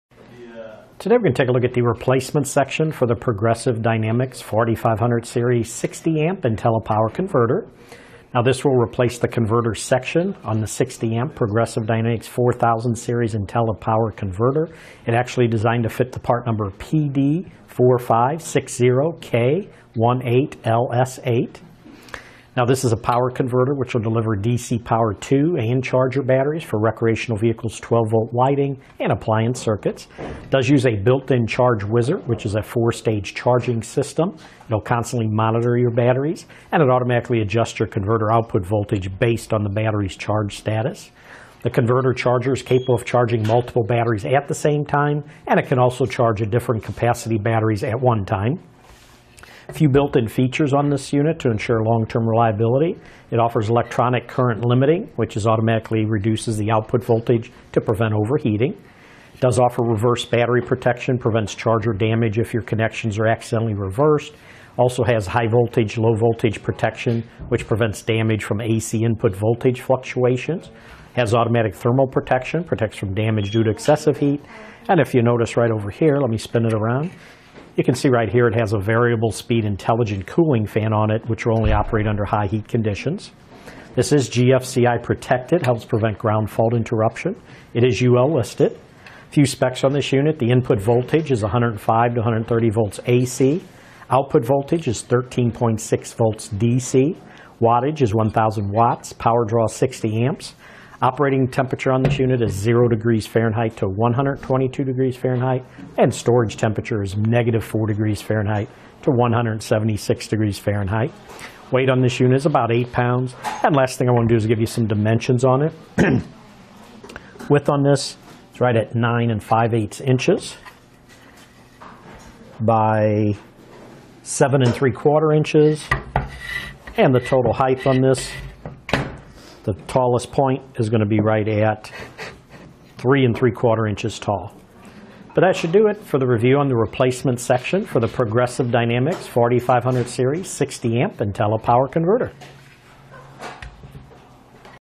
etrailer | Review of Progressive Dynamics - RV Converters - PD4560CSV
Click for more info and reviews of this Progressive Dynamics RV Power Converter:

Today we're going to take a look at the replacement section for the Progressive Dynamics 4500 Series 60 Amp Inteli-Power Converter. Now this will replace the converter section on the 60 Amp Progressive Dynamics 4500 Series Inteli-Power Converter. It's actually designed to fit the part number PD4560K18LS8. Now this is a power converter, which will deliver DC power to and charge your batteries for recreational vehicle's 12V lighting and appliance circuits. It does use a built-in Charge Wizard, which is a four stage charging system. It'll constantly monitor your batteries and it'll automatically adjust your converter output voltage based on the battery's charge status.The converter charger is capable of charging multiple batteries at the same time, and it can also charge different capacity batteries at one time.

A few built-in features on this unit to ensure long-term reliability, it offers electronic current limiting, which automatically reduces the output voltage to prevent overheating. It does offer reverse battery protection, prevents charge or damage if your connections are accidentally reversed. Also has high voltage, low voltage protection, which prevents damage from AC input voltage fluctuations. Has automatic thermal protection, protects from damage due to excessive heat. And if you notice right over here, let me spin it around, you can see right here it has a variable speed intelligent cooling fan on it, which will only operate under high heat conditions.This is GFCI protected, helps prevent ground fault interruption.

It is UL listed. A few specs on this unit, the input voltage is 105V to 130V AC. Output voltage is 13.6V DC. Wattage is 1,200 watts. Power draw, 60 amps.

Operating temperature on this unit zero degrees fahrenheit to 122 degrees fahrenheit. And storage temperature is negative four degrees fahrenheit to 176 degrees fahrenheit. Weight on this unit is about eight pounds. And last thing I want to do is give you some dimensions on it. Width on this is right at 9-5/8" by 7-3/4".

And the total height on this, the tallest point, is going to be right at 3-3/4" tall. But that should do it for the review on the replacement section for the Progressive Dynamics 4500 Series 60 Amp Inteli-Power Converter.